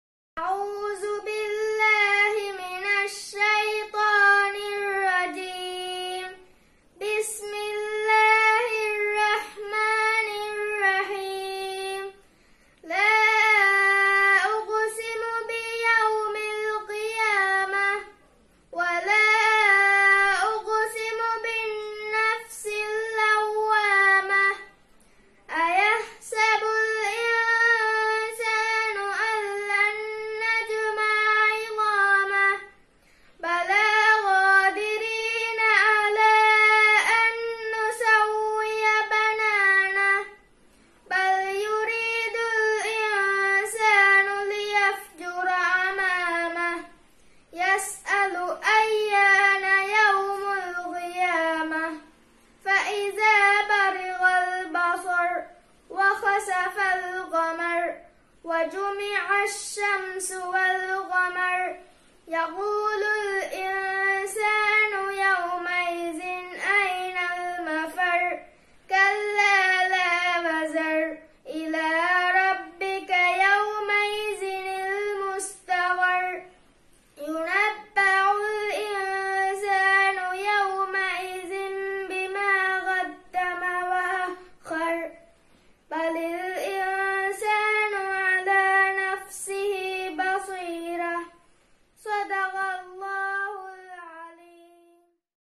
287 AO Quraan Competition 1441 (Aishath Yooha Binth Hassan Athif)
AO Quraan Competition 1441
Participant : 287
Aishath Yooha Binth Hassan Athif (Under 12)
Moonlight R.Innamaadhoo